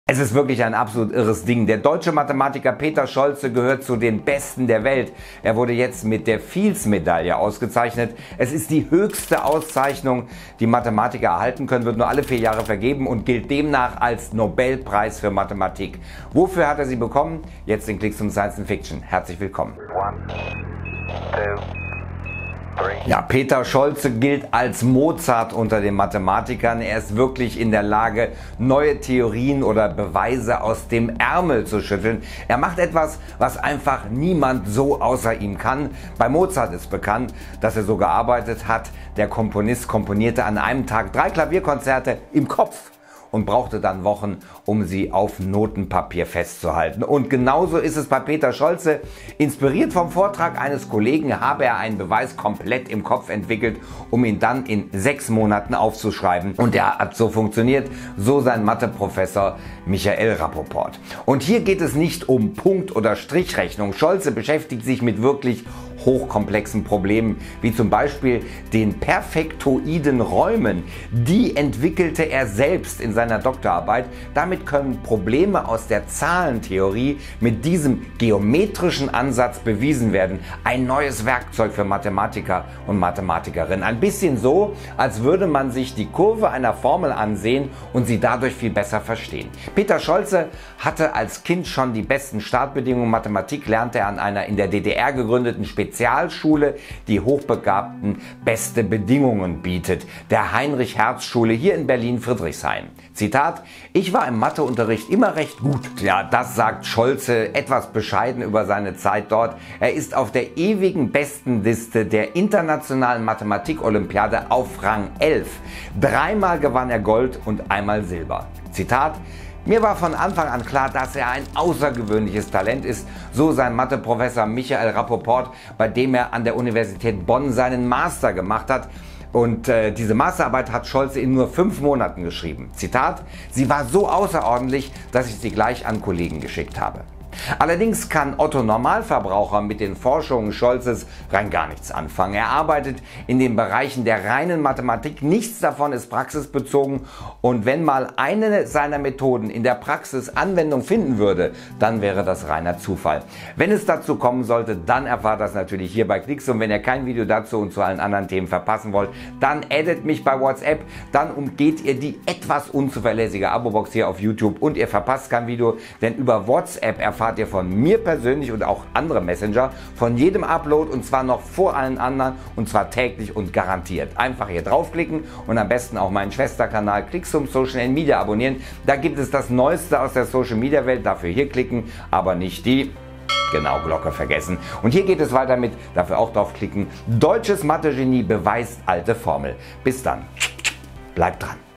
Deutscher Mathematiker bekommt “Nobelpreis für Mathematik” - Clixoom Science & Fiction
Der deutsche Mathematiker Peter Scholze wurde jetzt mit der Fields Medaille ausgezeichnet.